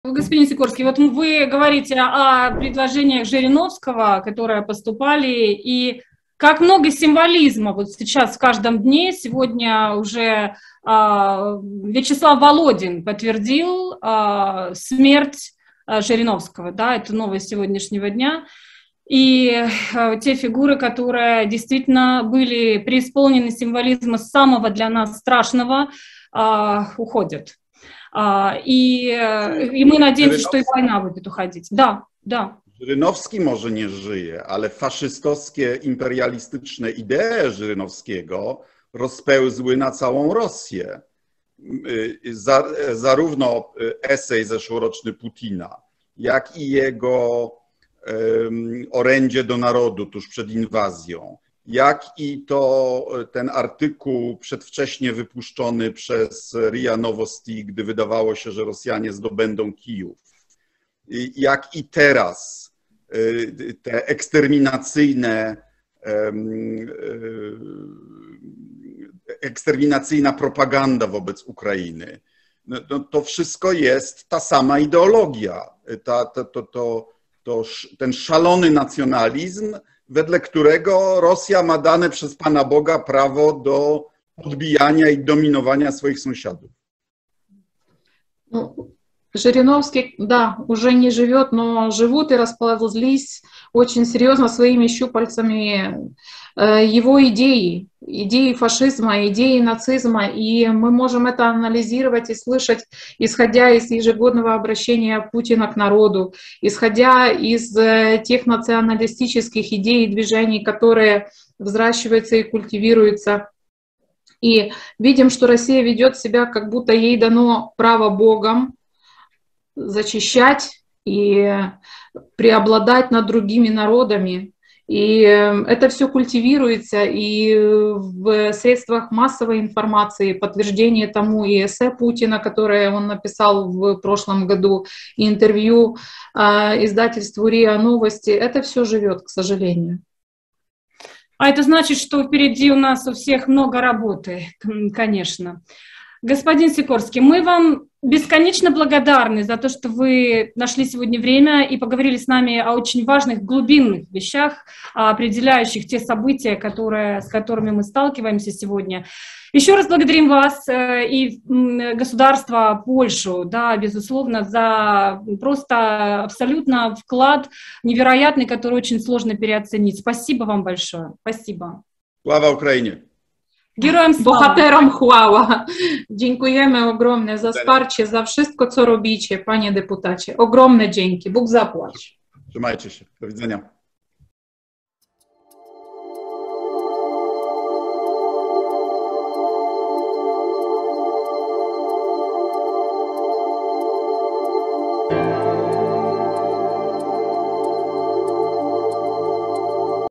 💬СІКОРСЬКИЙ пояснив, чому смерть Жириновського не допоможе Україні - війна, рф - Україна 24
Колишній Міністр закордонних справ Польщі Радослав Сікорський коментує смерть Володимира Жириновського та розповідає, що його смерть не забере з собою у могилу його фашистські ідеї.